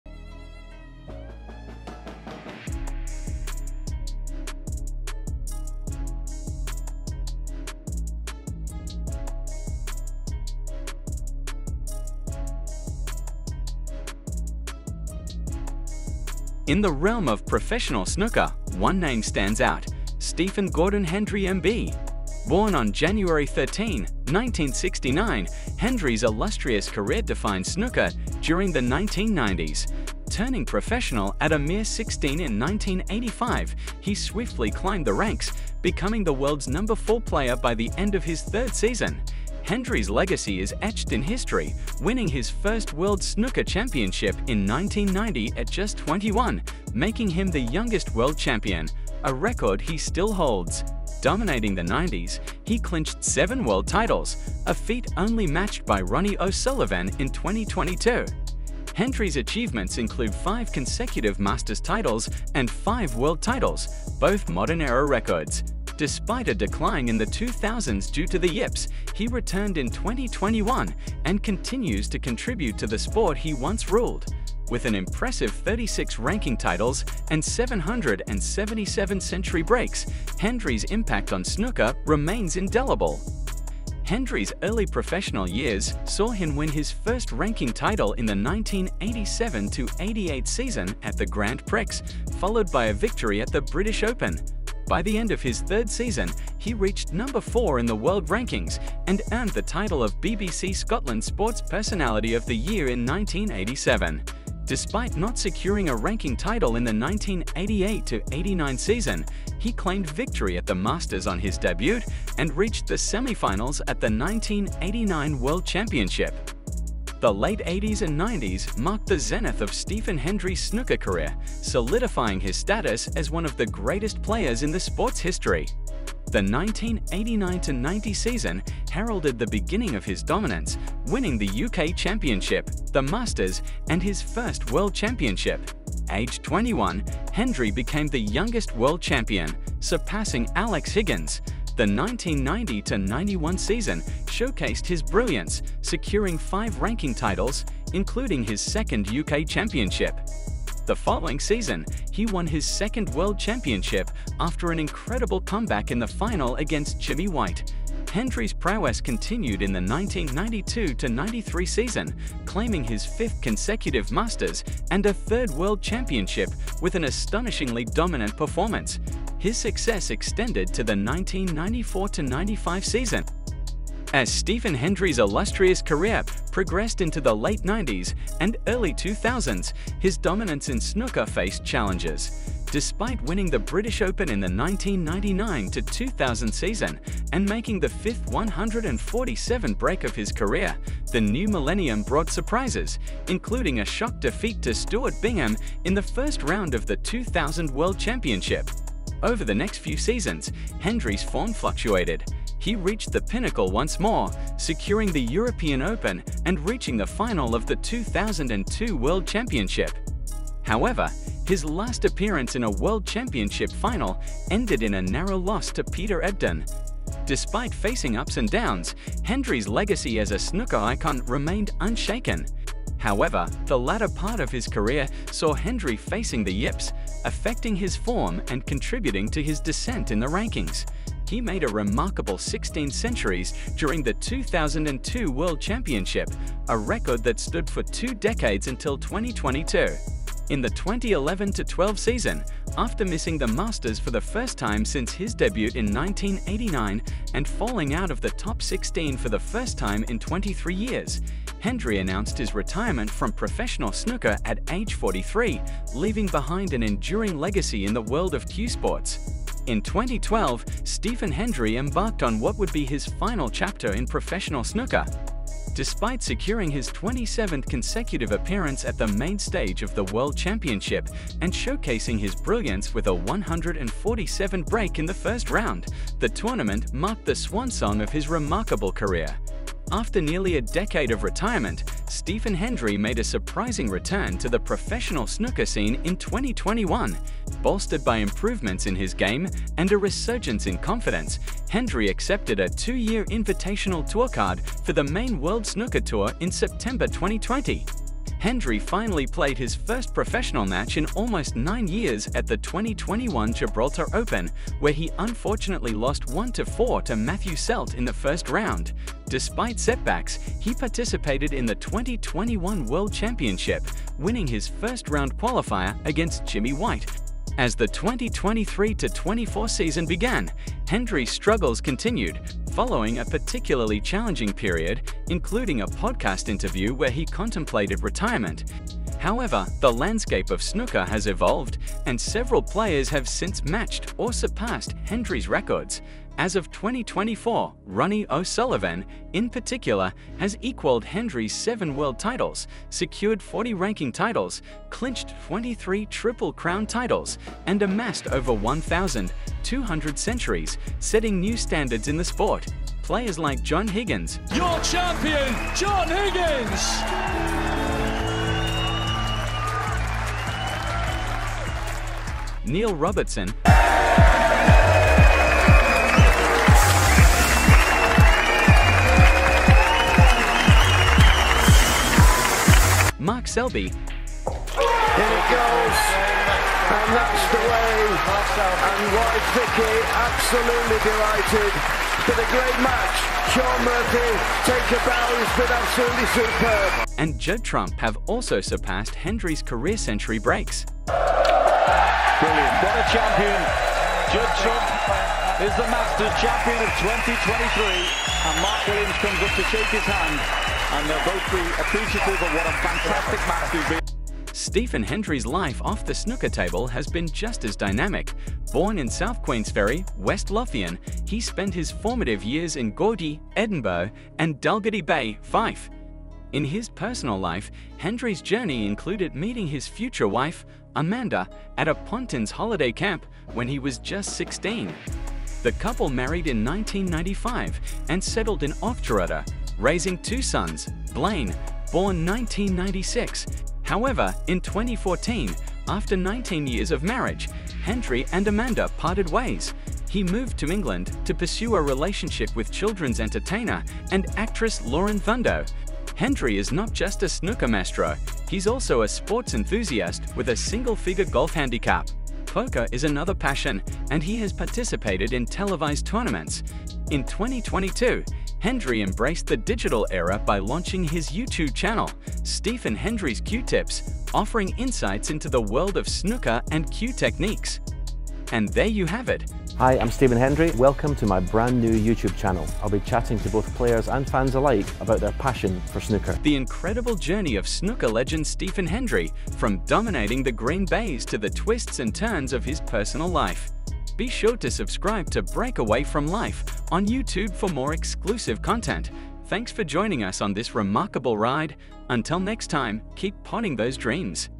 Stephen Hendry : The Snooker Symphony | A Journey through Triumphs, Records, Life
"This comprehensive YouTube video 📹 delves into the extraordinary life and career of Stephen Gordon Hendry MBE, a monumental figure in professional snooker 🎱. The host takes us on a journey, beginning with Hendry's early years when he turned professional at 16 🌟 and rapidly rose to prominence, becoming the youngest World Snooker Champion at 21 🏆. The video chronicles his domination in the 1990s, highlighting his seven world titles 🏅, unprecedented achievements in the Masters and world championships 🌍, and his remarkable record of 777 century breaks 💥.

The narrative then explores Hendry's challenging period in the 2000s, marked by fluctuating form and struggles with the yips 😓, yet underscoring his resilience 💪 and continued contributions to the sport. The host also covers Hendry's retirement in 2012 🛑, followed by his surprising comeback in 2021 🔙, detailing the highs and lows of his return to professional snooker.

Beyond his snooker prowess, the video provides insights into Hendry's personal life, including his relationships ❤️, hobbies like golf 🏌️ and poker ♠️, and his support for football clubs ⚽. It also touches upon financial challenges 💸 and his adaptation to the digital age with his YouTube channel 💻.

This video is a tribute to Stephen Hendry's legacy, celebrating his enduring impact on snooker and offering a candid look at the life of a sports legend.